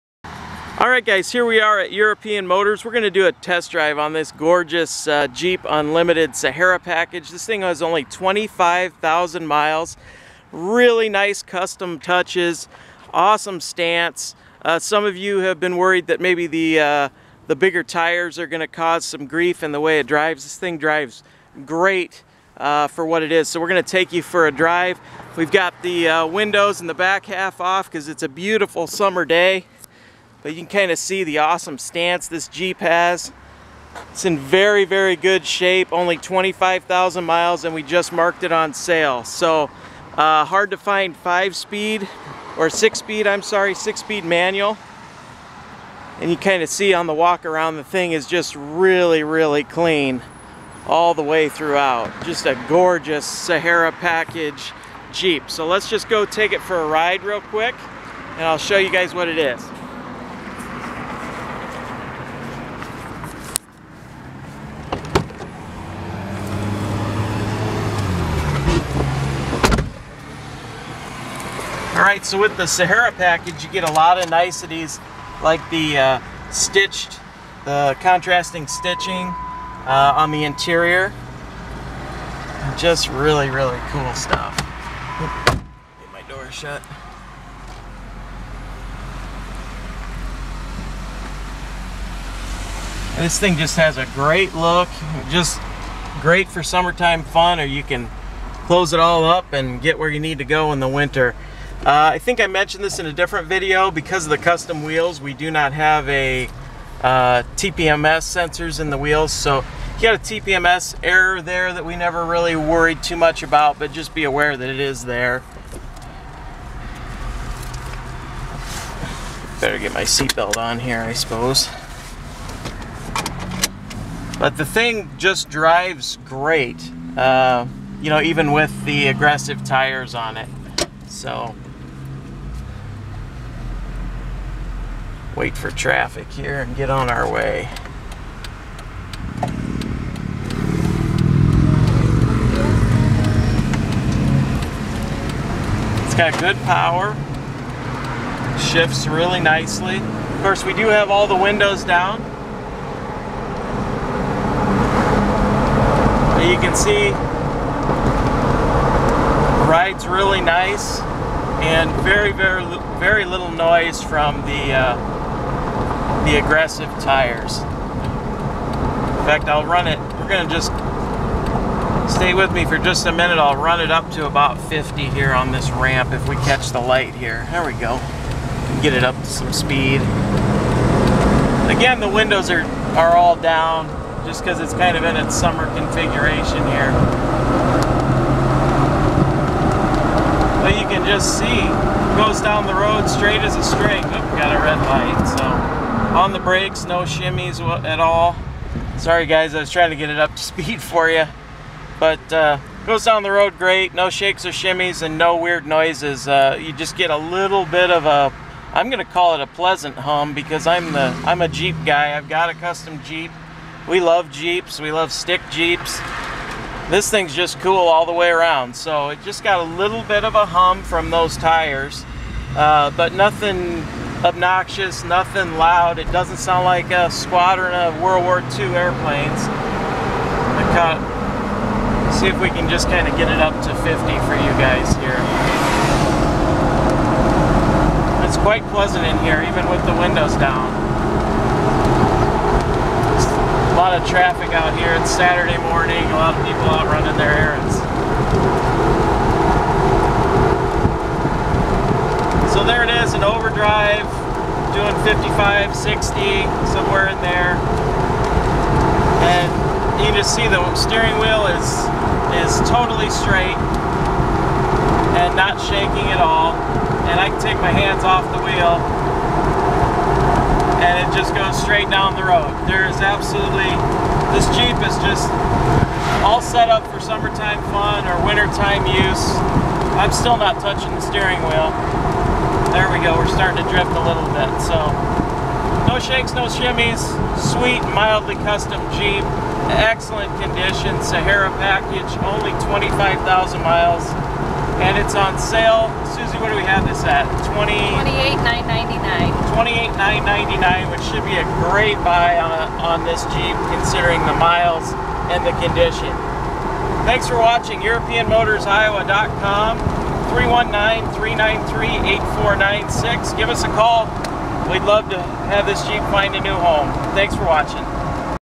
2014 Jeep Wrangler Umlimited Sahara 4x4 Test Drive
Here we take the 2014 Jeep Wrangler Unlimited Sahara 4x4 for a test drive.  This car is for sale at our dealership.